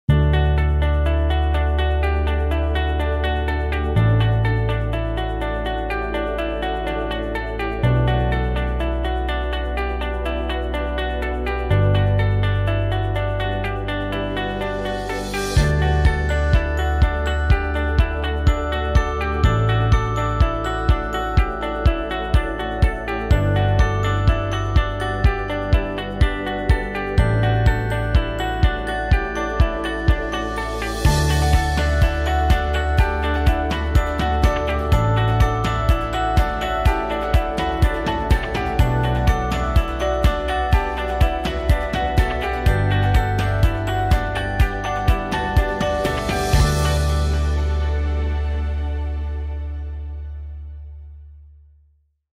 Joran Vliegen & Sander Gillé visit AZ Monica Hospital
A lovely surprise 😍🇧🇪
@sandergille and @joranvliegen visited the AZ Monica Deurne this morning and cheered up some children.
A little impression of this morning! 📽🙌